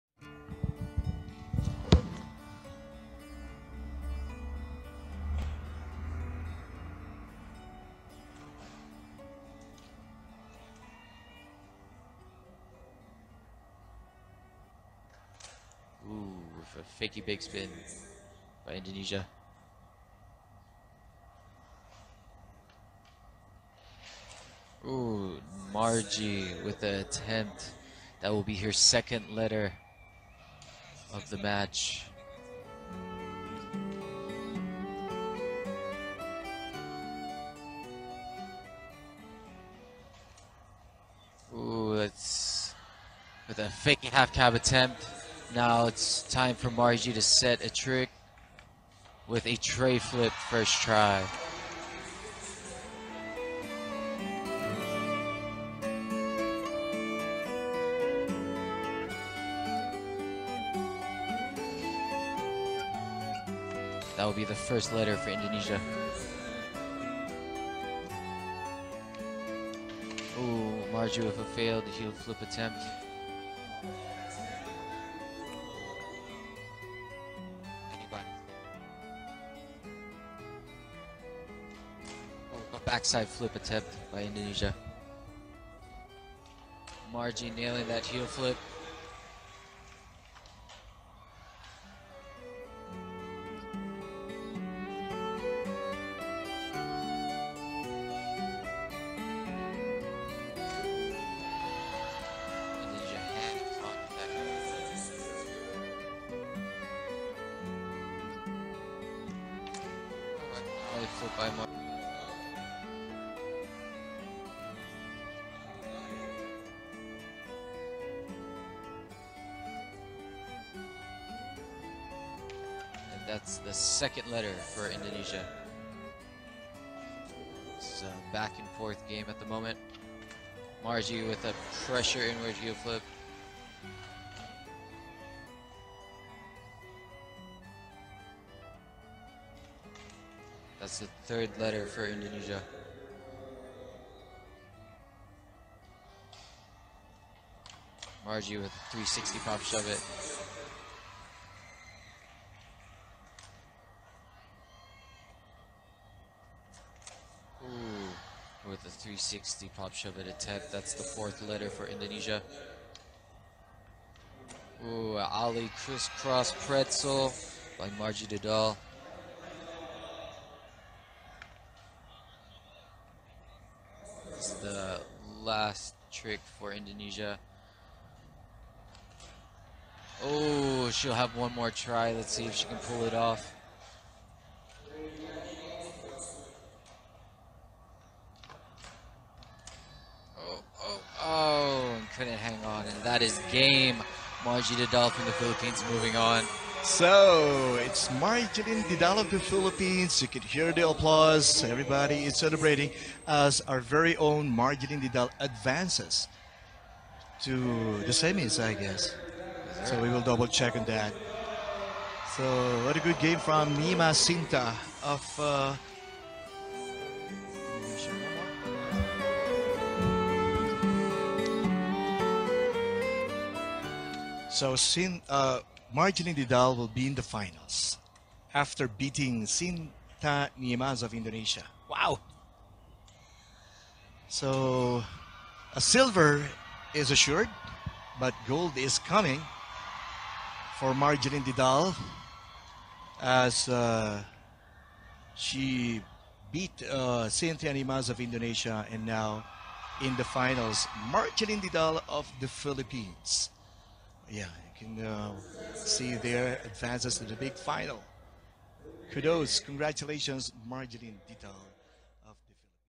MARGIELYN DIDAL- SEMI-FINALS WINNER IN SEA GAMES 2019 SKATEBOARDING
Margielyn Didal won against Nyimas Cinta in SEA Games 2019 Skateboarding Semi-finals.